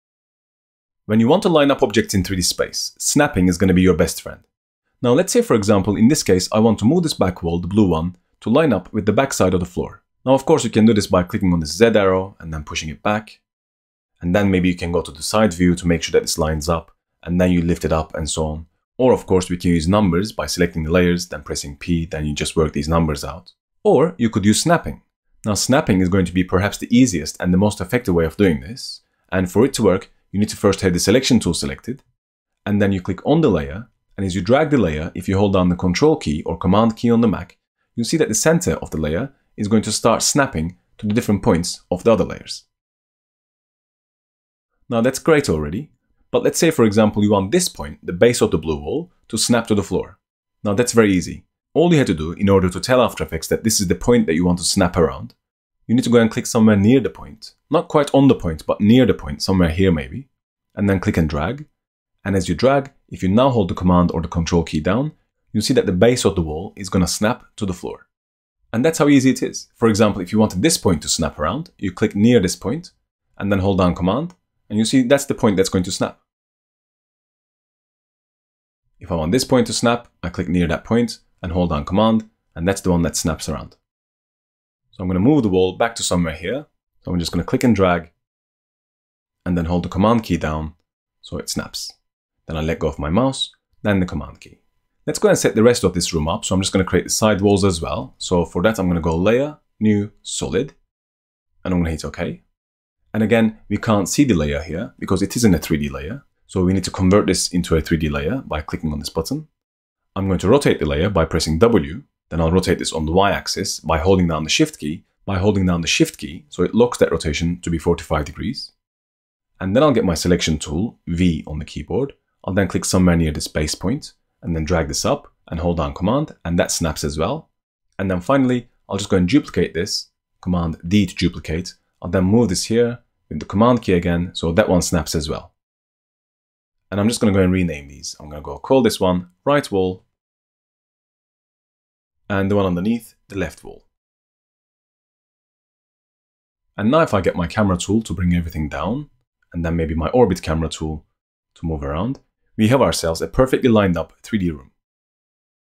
After Effects Hidden Trick - How To Snap 3D Layers in After Effects
One of the most important things about working in 3D in After Effects is to position the layers precisely. In this tutorial, you will learn how to use the standard selection tool to snap 3D layers to each other.

If you want to learn more about After Effects, check out our After Effects Masterclass here:

To see all other After Effects tutorials and courses, visit our After Effects training section here: